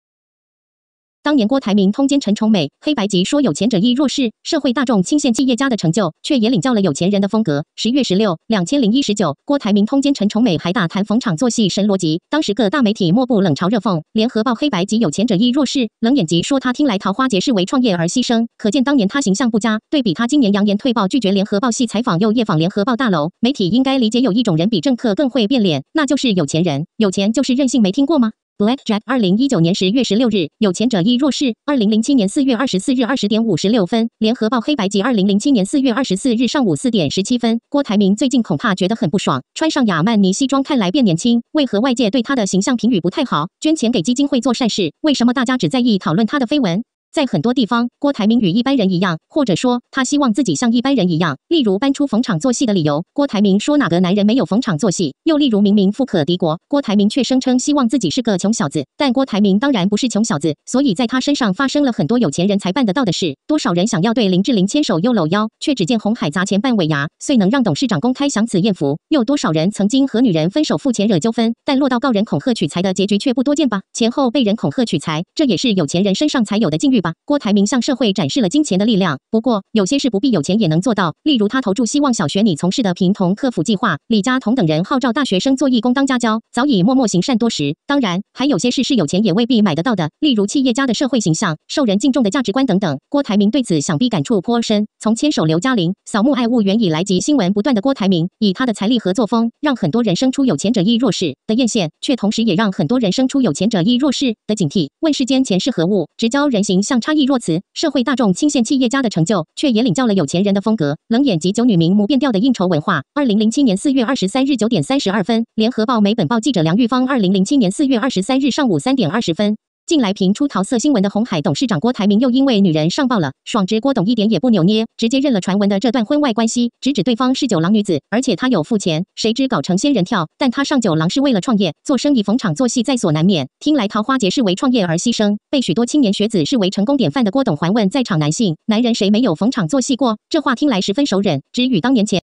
當年郭台銘通姦陳崇美，黑白集說有錢者亦若是？社會大眾欽羨企業家的成就，卻也領教了「有錢人」的風格！
10月 16, 2019
郭台銘通姦陳崇美還大談「逢場作戲」神邏輯，當時各大媒體莫不冷嘲熱諷，聯合報黑白集「有錢者亦若是？」、冷眼集說他「聽來桃花劫是為創業而犧牲」，可見當年他形象不佳，對比他今年揚言退報拒絕聯合報系採訪又夜訪聯合報大樓，媒體應該理解有一種人比政客更會變臉：

那就是有錢人。

「有錢就是任性」沒聽過嗎？

Blackjack 2019/10/16
江岷欽教授形容的最好：郭台銘你的政治導師叫輸不起
有錢者亦若是？
2007/04/24 20:56
【聯合報／黑白集】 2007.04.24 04:17 am